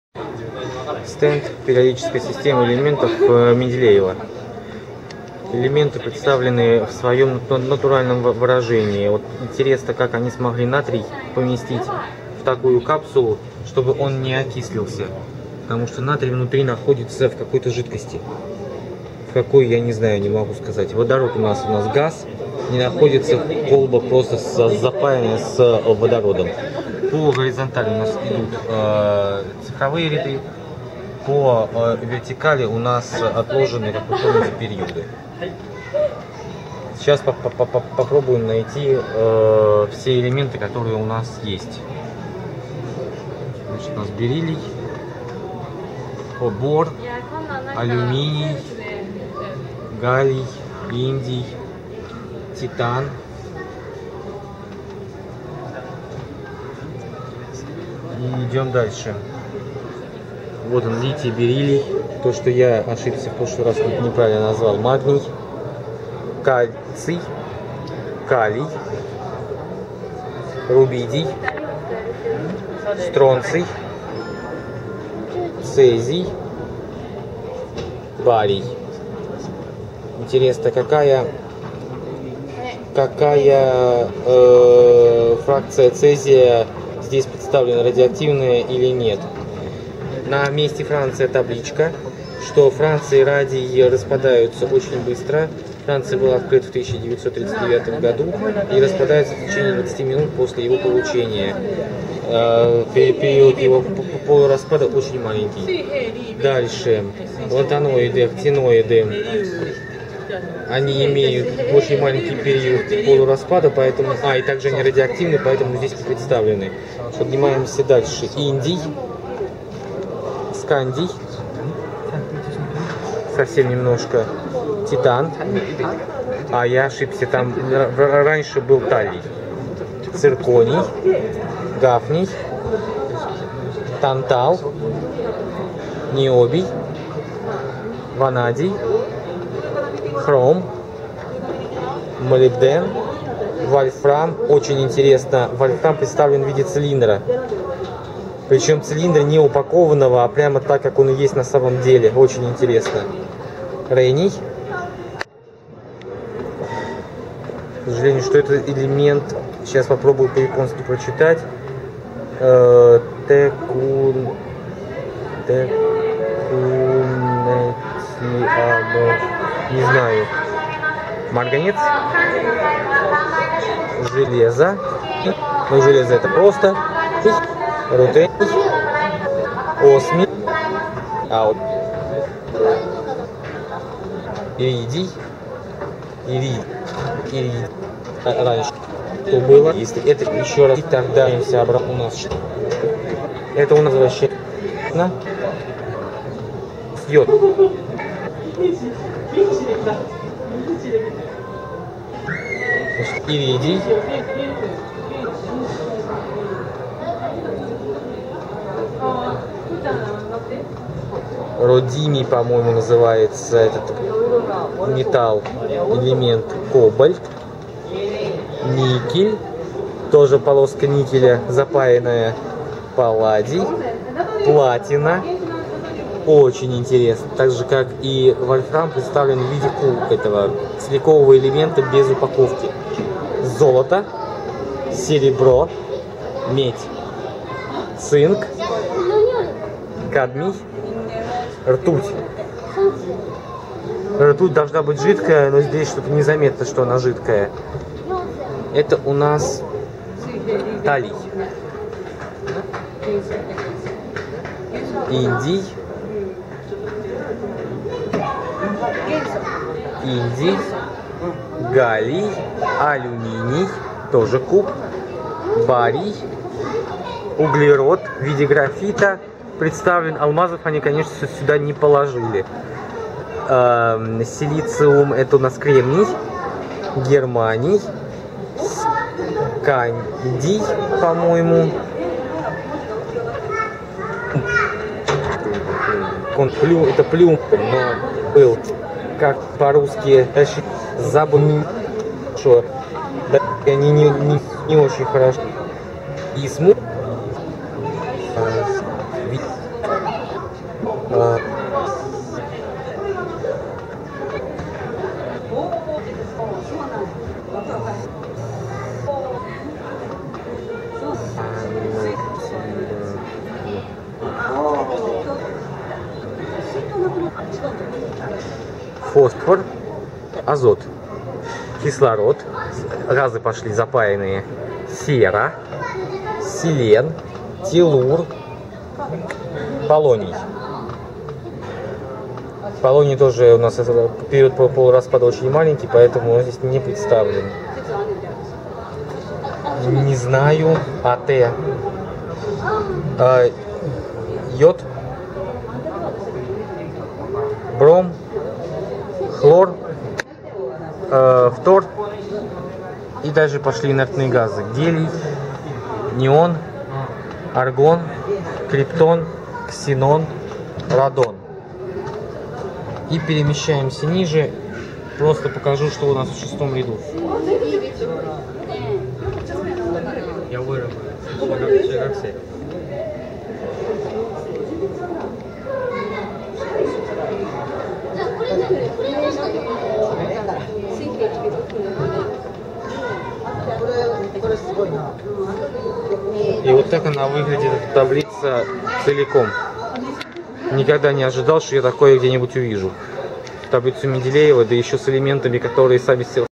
Япония за 14 дней - День 12 - Музей науки Осака - Периодическая система элементов из них самих
Периодическая система элементов включает все химические элементы, известные на сегодняшний день.

Путешествуя по Японии, обнаружил, что музее науки в Осаке есть стенд с таблицей периодических элементов и в нем каждый элемент представлен сам собою. Металлы - слитком металла, газ запаян в колбу. На стенде, по понятным причинам, отсутствуют радиоактивные элементы и элементы с малым периодом полураспада.